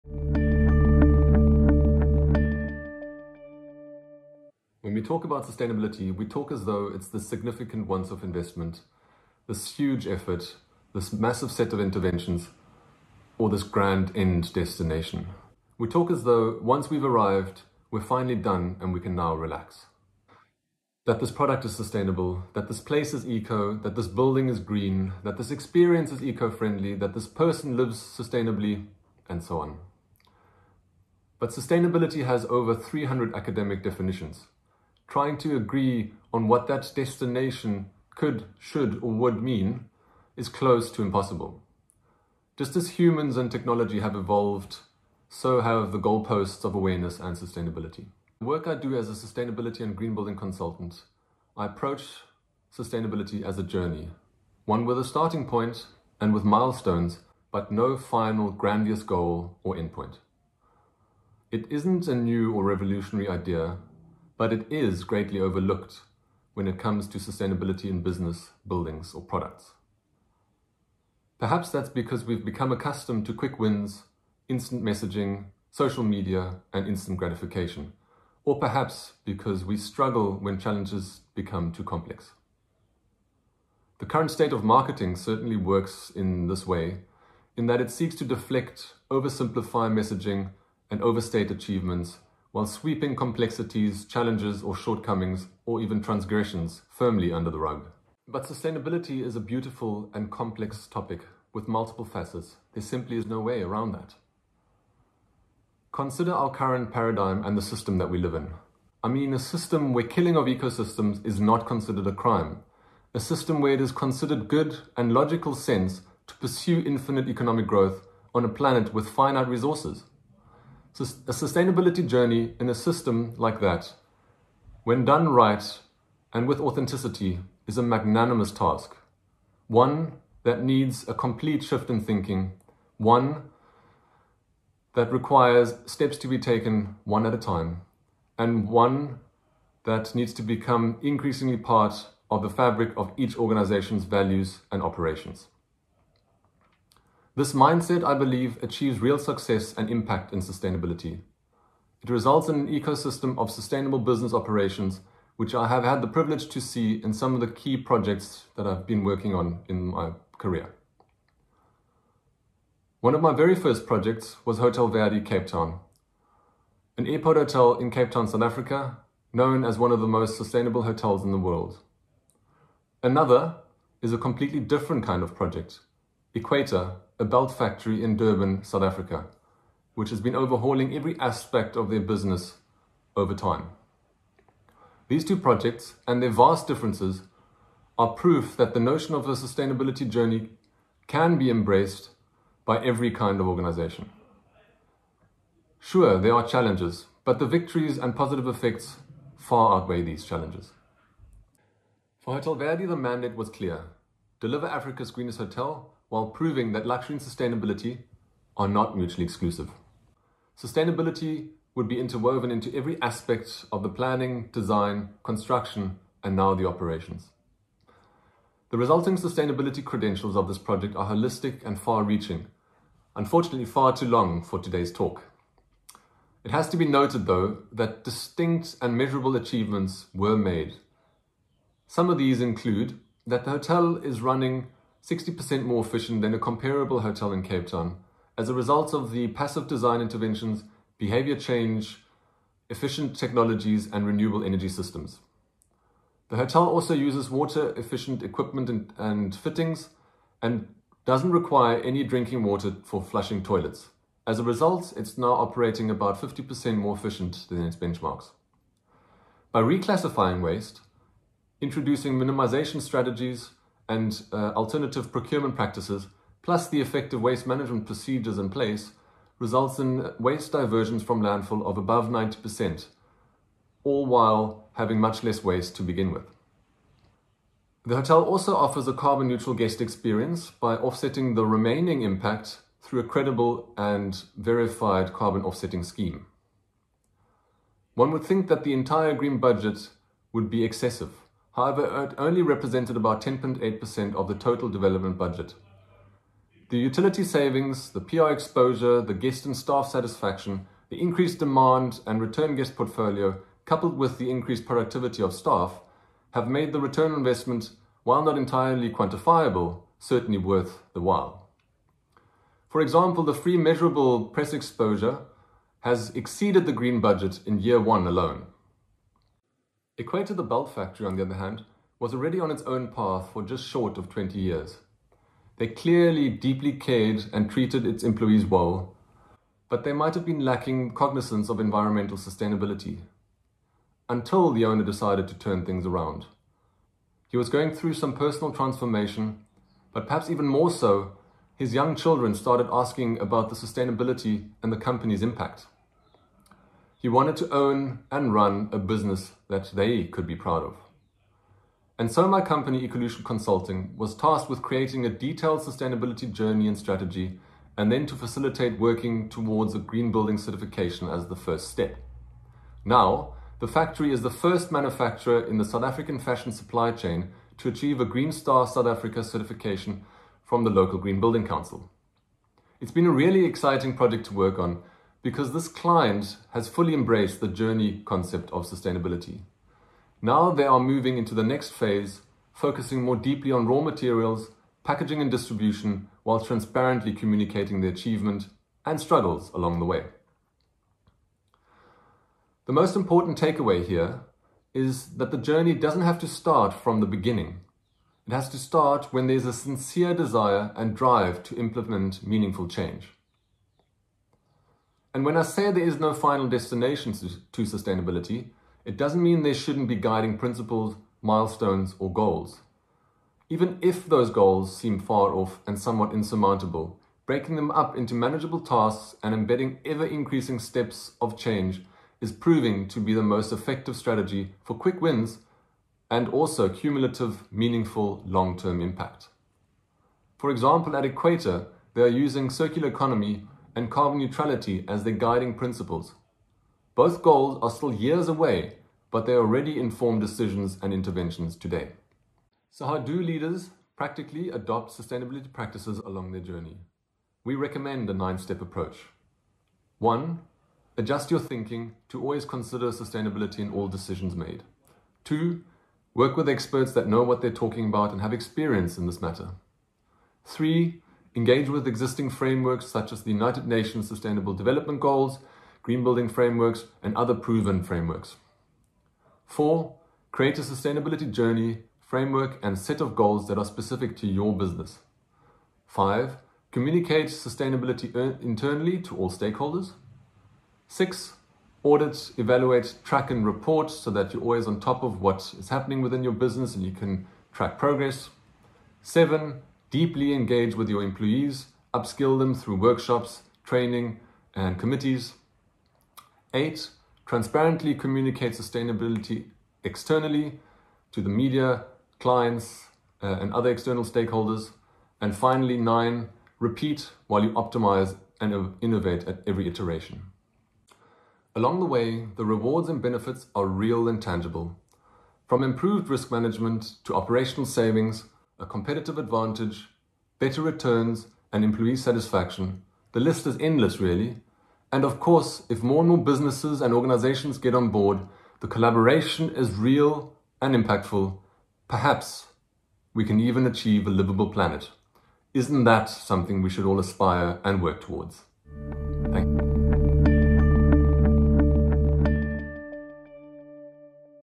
The Journey of Sustainability | André Harms | TEDxSUMAS
We are currently using a concept of indefinite growth in an eco-system with finite resources. There are 300 academic definitions for sustainability - the term means something different to different people or groups all-encompassing a long-term view.
Reaching a truly sustainable, regenerative business is a journey with no end but with a start and specific milestones on the way. While the ultimate goal seems unreachable, it is very effective to break it down into manageable tasks. This will create long-term meaningful impacts. Efforts towards regeneration will not only have measurable effects on lower costs, increased customer satisfaction, staff productivity, and free marketing or PR are just a few benefits that accompany sustainable businesses. André is the founder of @ecolutionconsulting , a sustainabilityand greenbuilding consulting firm based in Cape Town. André is an electro-mechanical engineer and qualified Renewable Energy Professional with extensive experience in design, project management, commissioning process implementation and auditing sites for sustainabilityprojects. He was a key player in the success of Africa’s Greenest Hotel, Hotel Verde, being the lead sustainabilityconsultant on the project.

Having spent 14 months in Antarctica as mechanical engineer and team leader of the South African National Antarctic overwintering expedition, he has a special interest in efficiency, functionality, serviceability and robustness of systems. With experience in Net Zero, EDGE, Green Star SA, LEED, project management and the implementation of the ASHRAE Guideline 0 commissioning process, André often performs multiple functions on projects. This talk was given at a TEDx event using the TED conference format but independently organized by a local community.